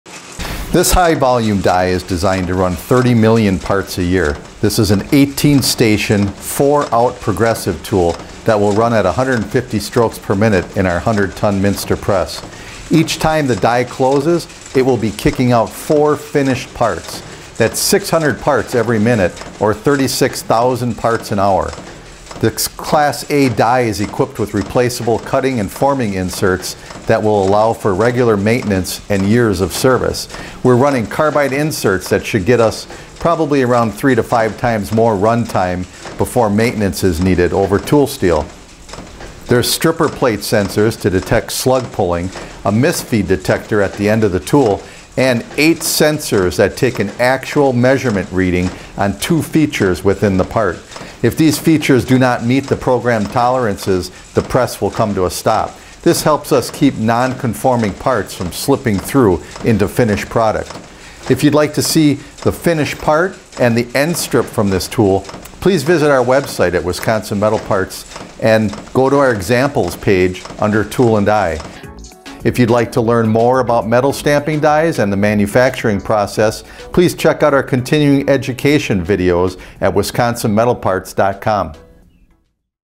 High Volume Progressive Die
This high-volume progressive stamping die is designed to run 30,000,000 parts per year to help meet increased demand and lower production costs.